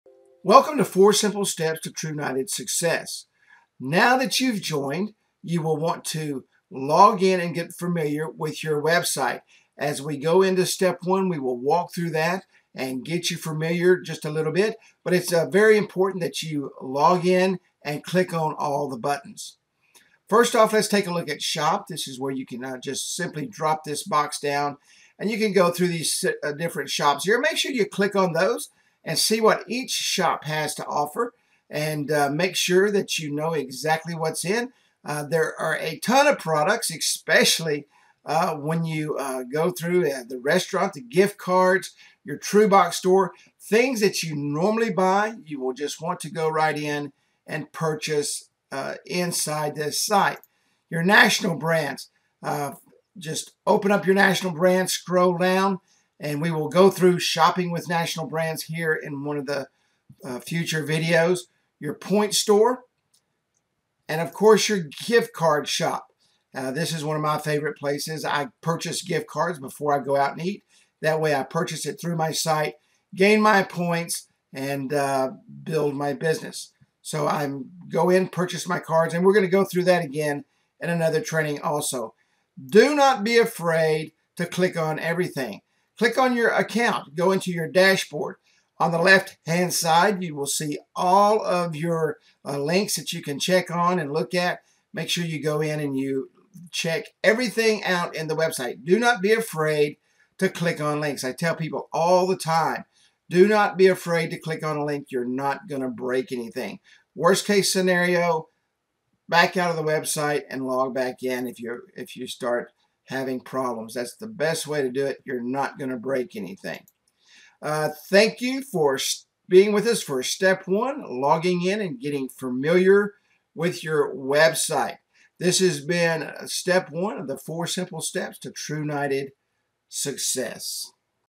Step 1. Login, Get Familiar with the Website - Trunited
4 Simple Steps To Trunited Success!
Step1. Login, Get Familiar with the Website

Getting familiar with the Trunited Website is very important and a key to you success. Do not be afraid to click on the buttons and explore. Knowledge is power and their is a lot of knowledge in your Trunited site